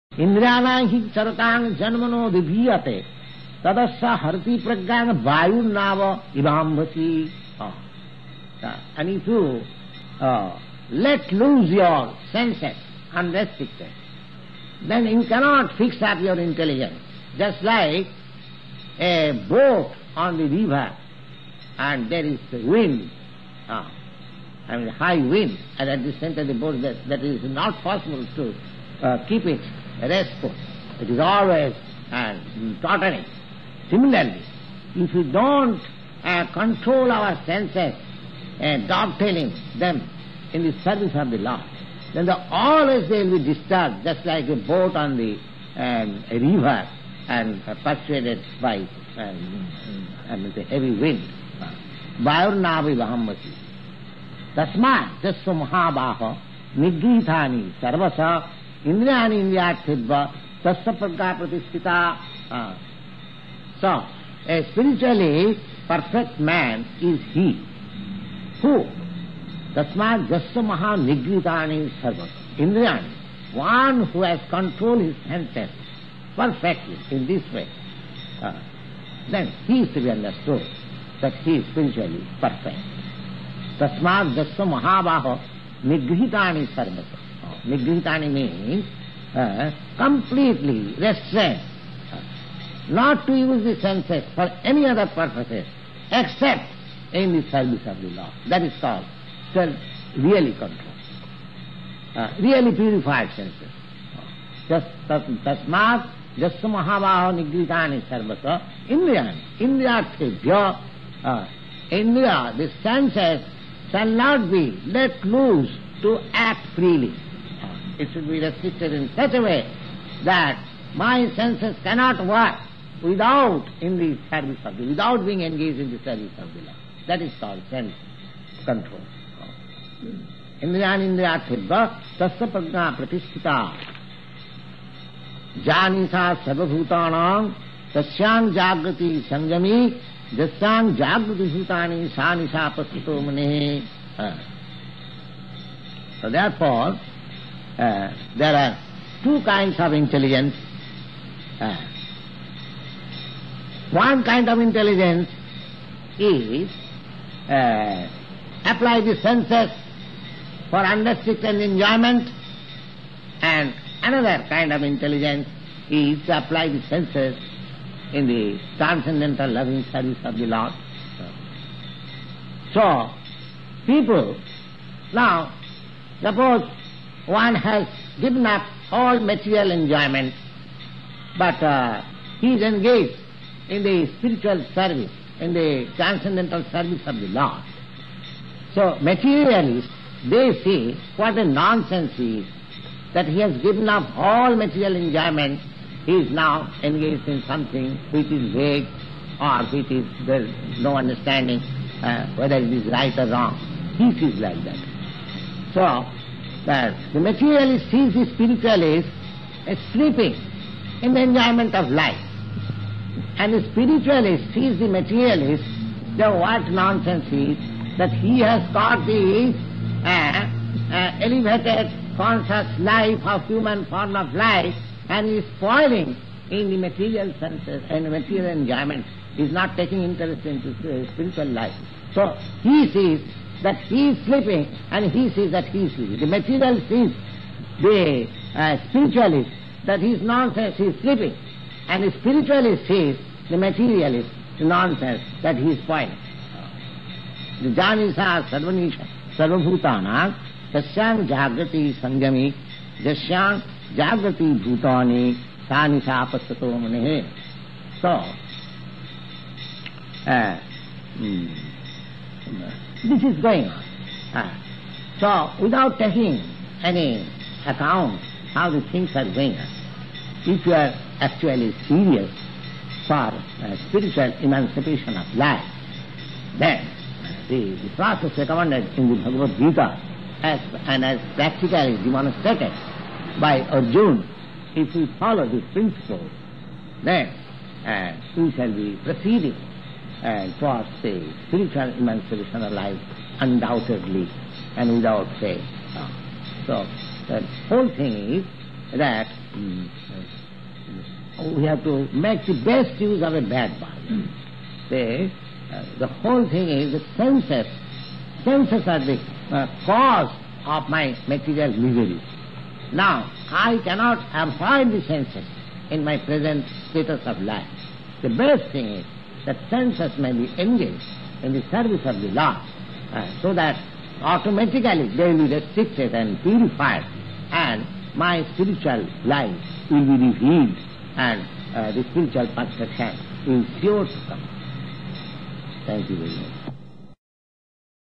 Two Kinds of Intelligence @ShrilaPrabhupadaShortLectures
indriyāṇāṁ hi caratāṁ
yan mano 'nuvidhīyate
tad asya harati prajñāṁ
vāyur nāvam ivāmbhasi

[Bg 2.67]

[As a boat on the water is swept away by a strong wind, even one of the senses on which the mind focuses can carry away a man's intelligence.]

tasmād yasya mahā-bāho
nigṛhītāni sarvaśaḥ
indriyāṇīndriyārthebhyas
tasya prajñā pratiṣṭhitā

[Bg 2.68]

[Therefore, O mighty-armed, one whose senses are restrained from their objects is certainly of steady intelligence.]

yā niśā sarva-bhūtānāṁ
tasyāṁ jāgarti saṁyamī
yasyāṁ jāgrati bhūtāni
sā niśā paśyato muneḥ

[Bg 2.69]

[What is night for all beings is the time of awakening for the self-controlled; and the time of awakening for all beings is night for the introspective sage.]

excerpt from a lecture given by His Divine Grace Abhay Charanaravinda Bhaktivedanta Swami Shrila Prabhupada on April 18, 1966 in New York